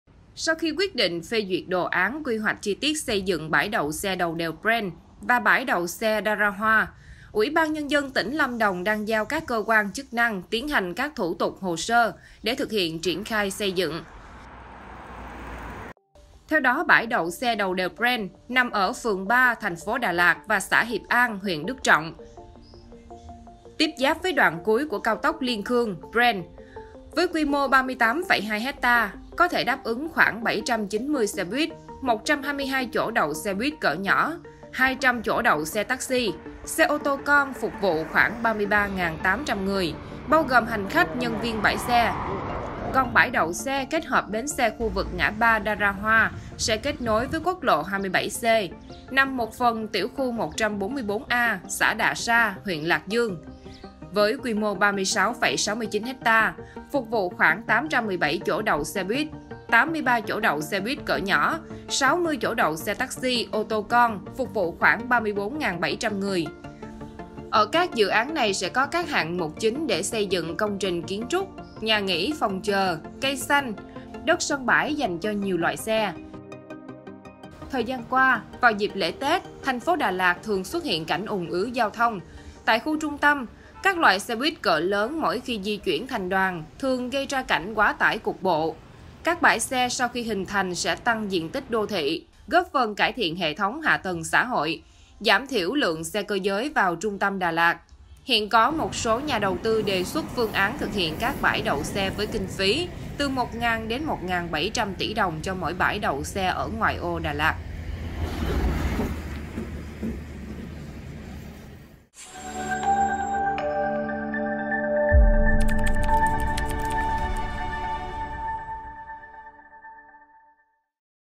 Đà Lạt sẽ có 2 bãi đậu xe lớn ở đèo Prenn và ngã ba Đarahoa
Sau khi quyết định phê duyệt đồ án quy hoạch chi tiết xây dựng bãi đậu xe đầu đèo Prenn và bãi đậu xe Đarahoa, UBND tỉnh Lâm Đồng đang giao các cơ quan chức năng tiến hành các thủ tục, hồ sơ để thực hiện triển khai xây dựng.